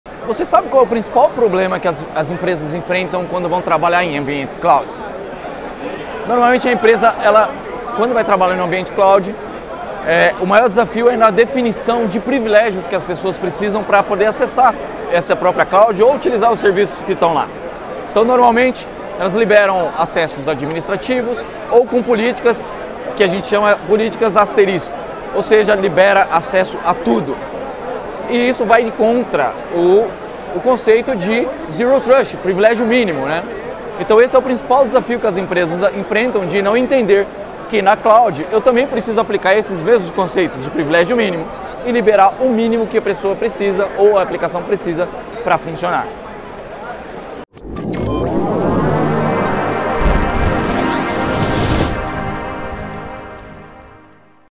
Ambiente Cloud - Short
A principal dificuldade em se trabalhar em ambientes de cloud em 1 minuto.

Nesse vídeo, nosso Head de Produtos, Ademir Diniz fala sobre os principais desafios em se trabalhar com zero trust em um ambiente de cloud.